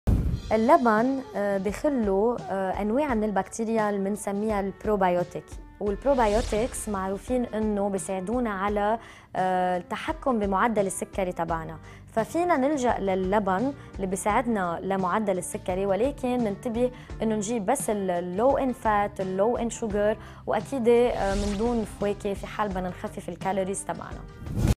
How Yogurt Affects Your Blood Sugar Level - Dietitian Christelle Bedrossian, LBC, Beirut-Lebanon
In this one minute tip, Dietitian Christelle Bedrossian talks about the effects of yogurt on blood sugar level.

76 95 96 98
Clinics in Adlieh and Jounieh
Lebanon